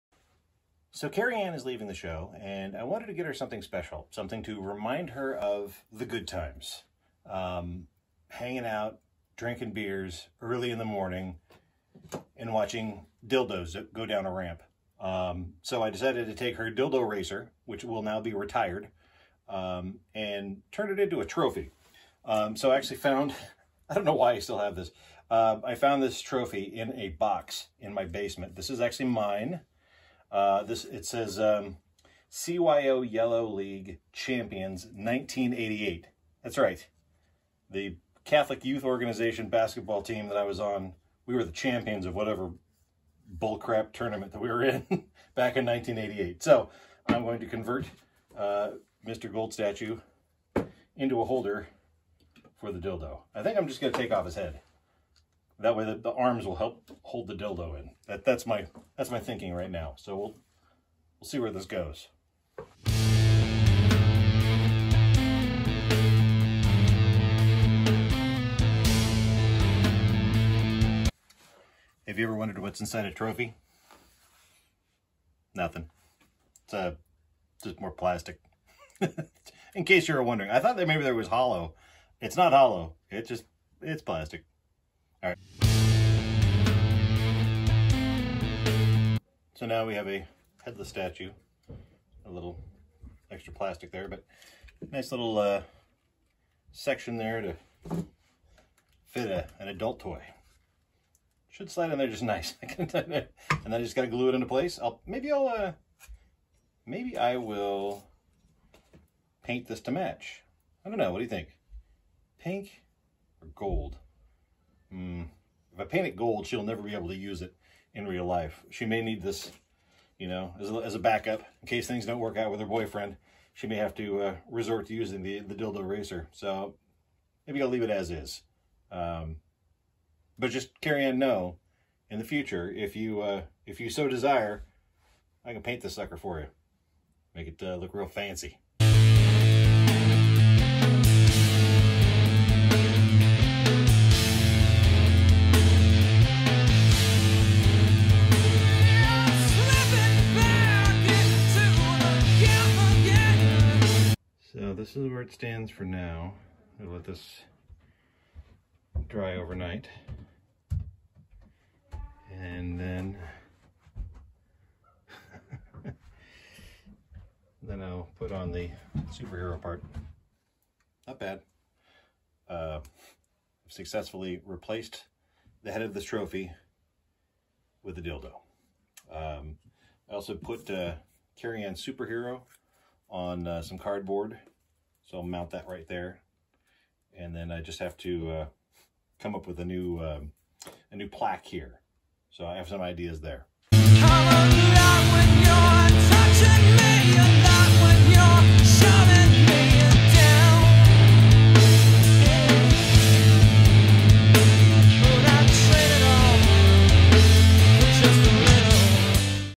Karianne's Dildo Racer
The process of turning Karianne's dildo derby racer into a statue for her to remember us by as she moves to Chicago.